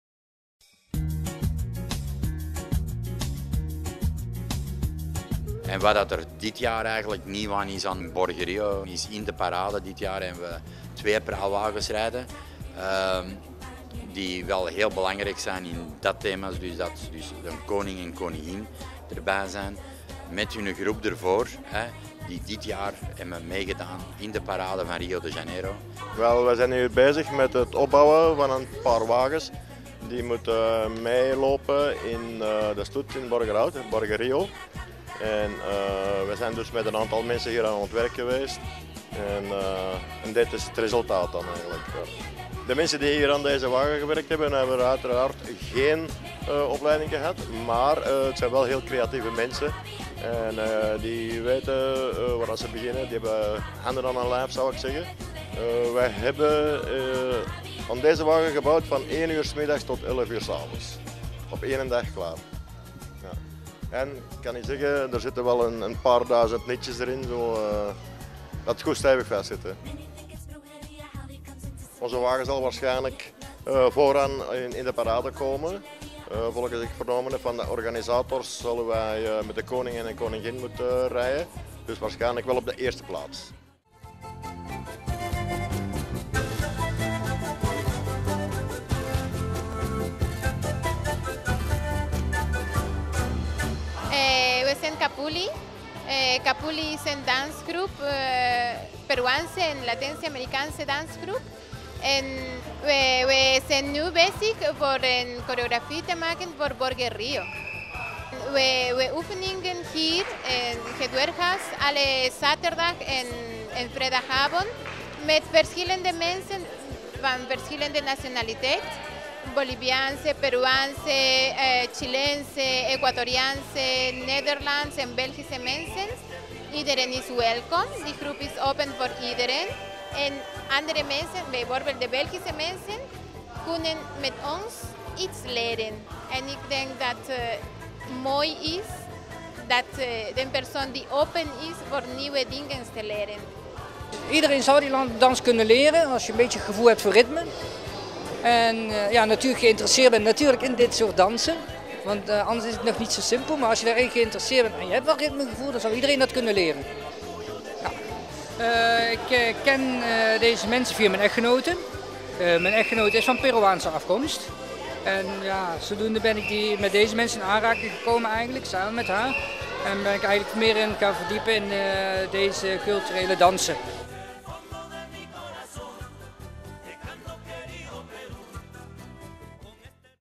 BorgerRio in volle voorbereiding
De voorbereidingen voor de derde editie van het BorgerRiofestival zijn volop aan de gang. De laatste hand wordt gelegd aan de twee praalwagens die de parade aanvoeren en waarop de koning en koningin van het festival meerijden. Hiervoor hebben vrijwilligers bestaande wagens aangekleed zodat ze perfect aansluiten bij het Zuid-Amerikaanse thema.

Dansgroep Kapuli brengt dan weer een choreografie van Peruaanse heupbewegingen, uitgevoerd door een bont gezelschap van dansers. Tijdens de repetities staan Chilenen, Peruanen en Belgen zij aan zij om de zuiderse danspassen onder de knie te krijgen.